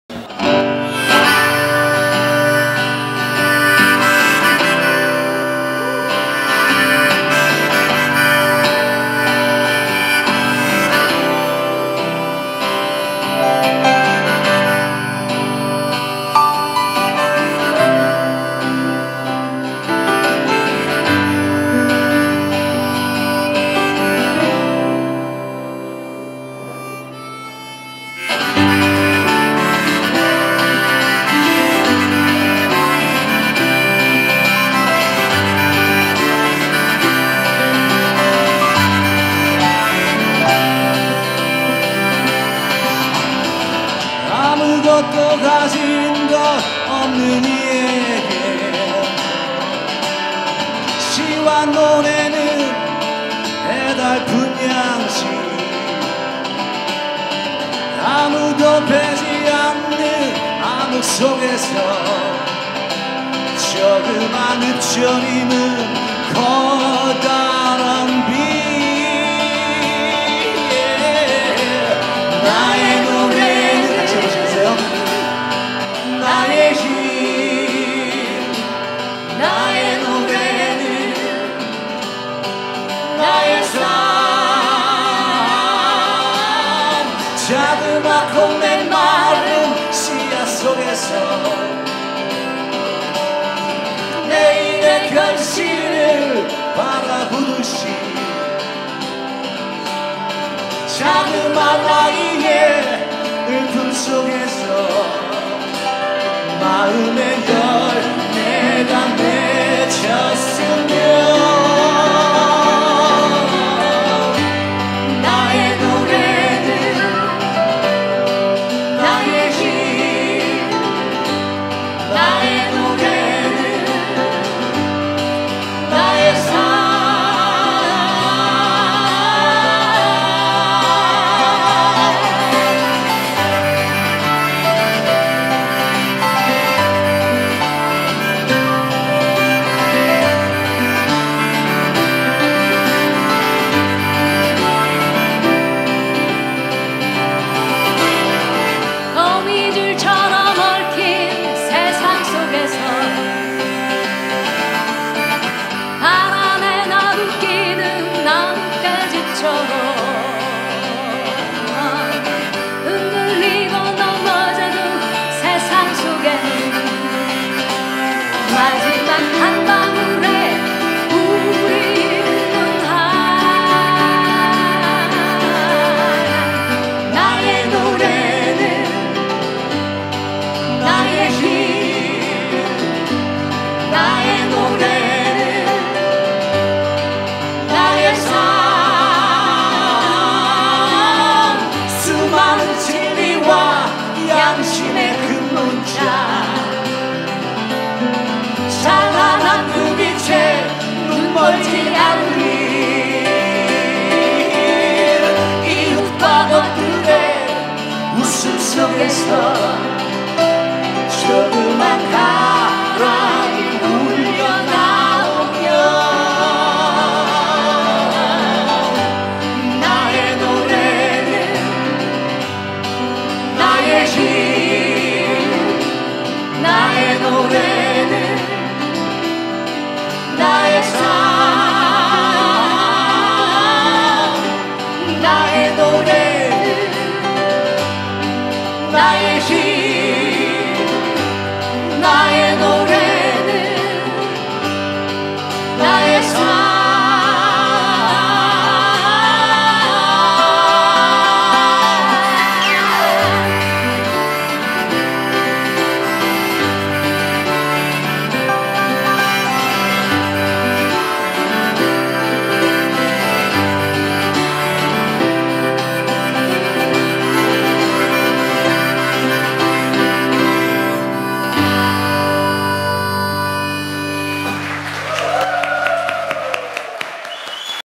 윤선애&정윤경 / 노래
제6회 제주 스피릿 콘서트
윤선애&정윤경의 노래 공개합니다